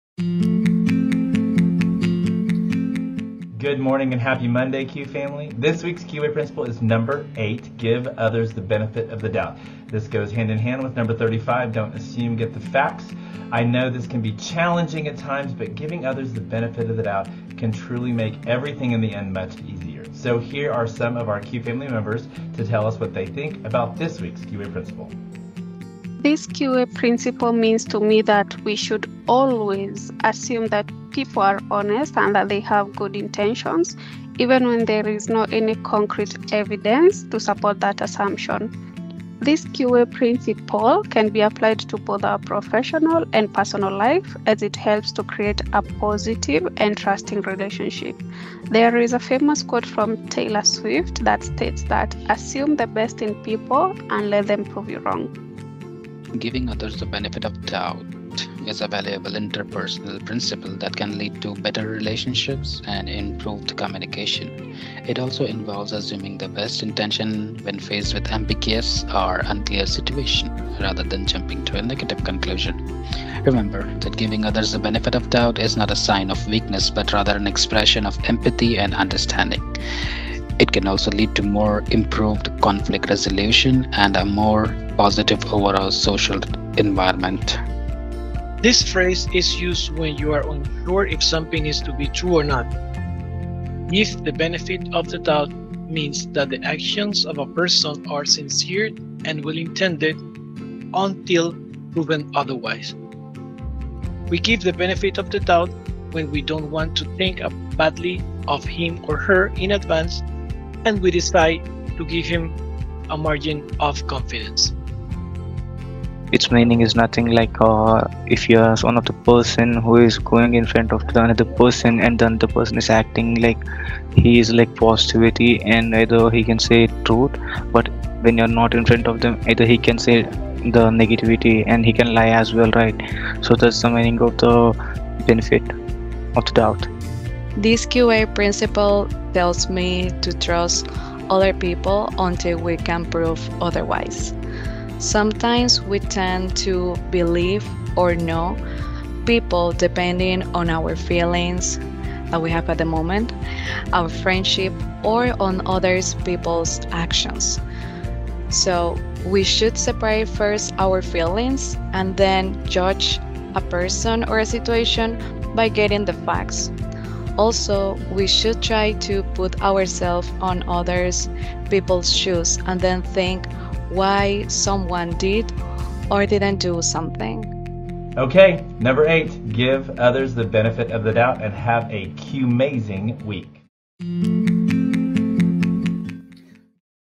🌟OnQ’s QWay Principle Number Eight: Give Others the Benefit of the Doubt
In our continuing series on the QWay Principles, we invite you to watch QFamily Members as they share their perspective on our eighth principle, “Give Others the Benefit of the Doubt”.

The QFamily continues to build a winning family through service and innovation! Learn more about the QFamily.

OnQ - Advancing Labor and Technology Solutions in Healthcare